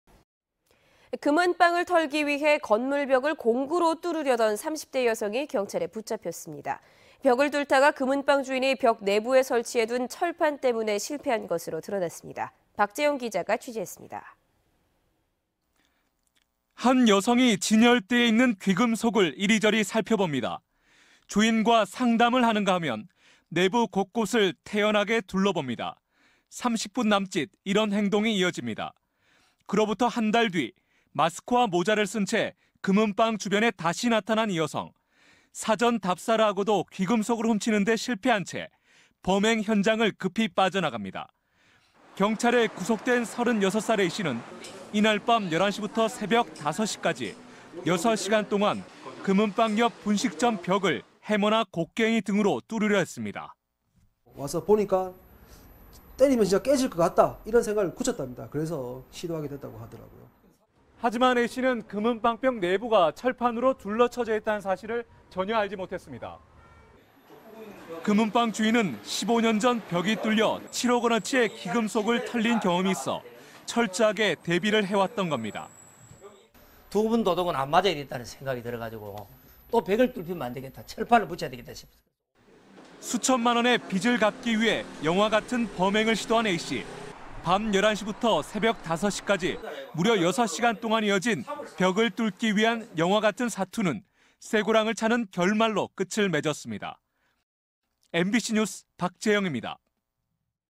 [대구MBC뉴스] 공구로 벽뚫고 금은방 절도 시도한 30대 여성
내용: 공구로 벽뚫고 금은방 절도 시도한 30대 여성 - 박재형 기자